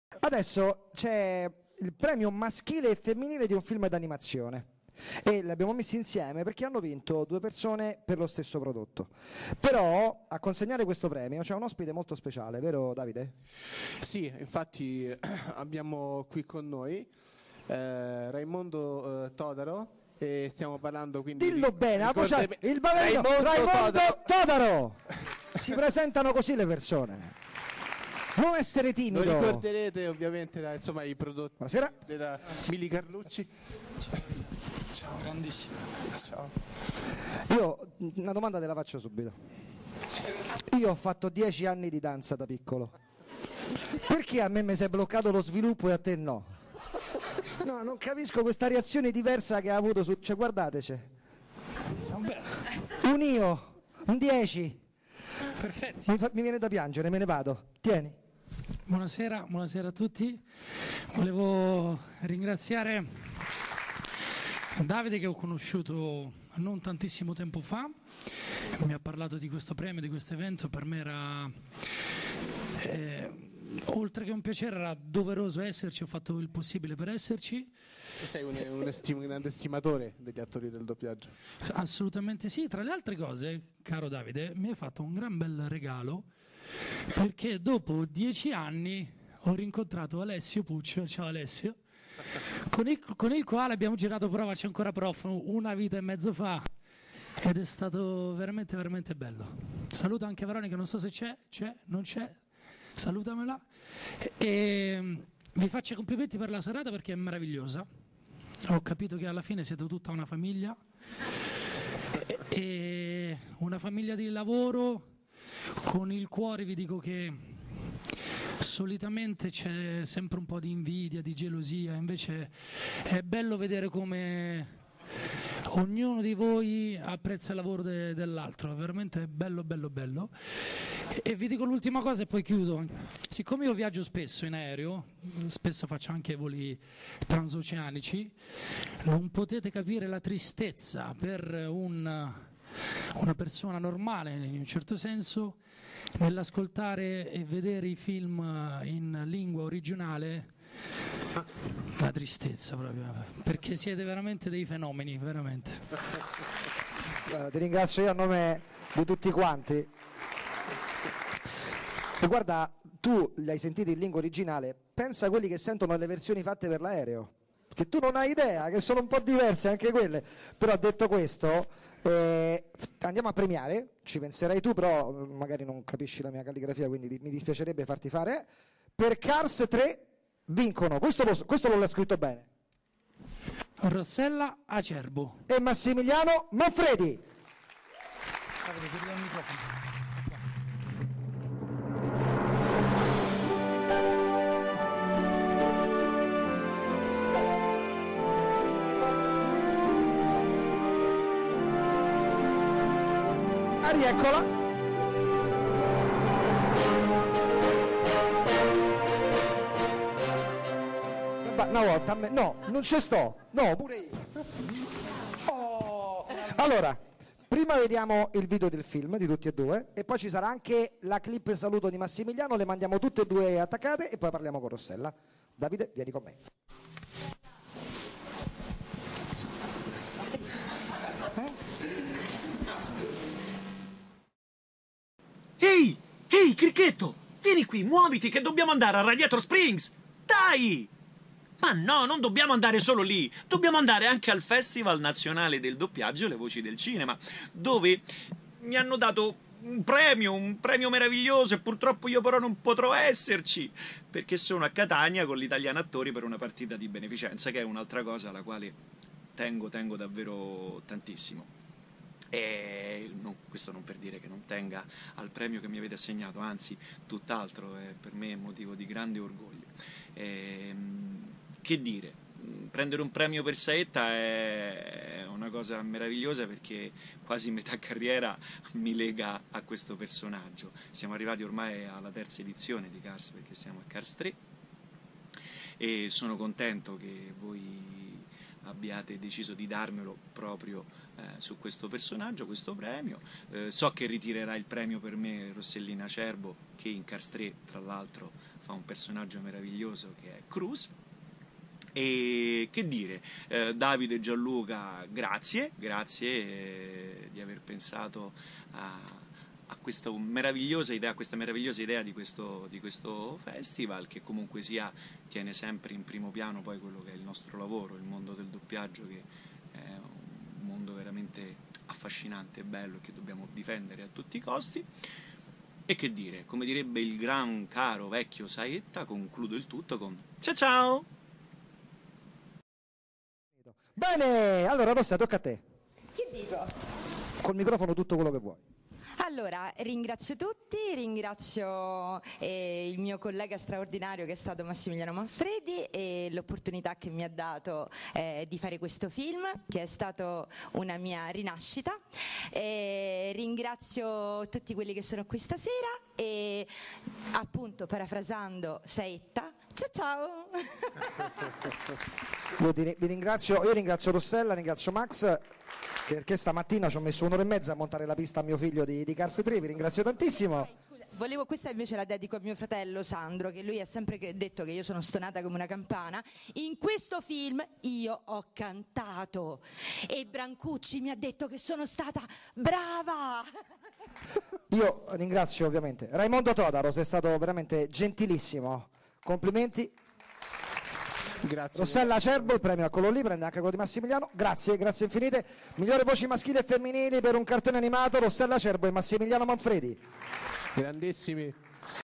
Rossella Acerbo e Massimiliano Manfredi - Festival "Le Voci Del Cinema" 2018
Festival Nazionale del doppiaggio “Le Voci Del Cinema” edizione 2018

Manifestazione del sabato 13 gennaio al Teatro Cassia di Roma.

Diretta radio dello speaker Jerry Longo col programma “Hasta La Vista” andata in onda su

Evento con partecipazione dei customing ufficiali di Star Wars dei gruppi nazionali 501st Italica Garrison e Rebel Legion Italian Base.

del promotore e teorico del doppiaggio Davide Pigliacelli (non vedente, sindrome di Behçet).

Serata presentata da Davide Pigliacelli e dal doppiatore Gianluca Crisafi.

Video e foto di Gianluca Pigliacelli.

Montaggi della società di doppiaggio Mediasintesi di Roma.

Nei prossimi giorni caricheremo altri video.(potete ottenere l’effetto stereo di questo video applicando uno spinotto adattatore alle vostre cuffie/casse del computer)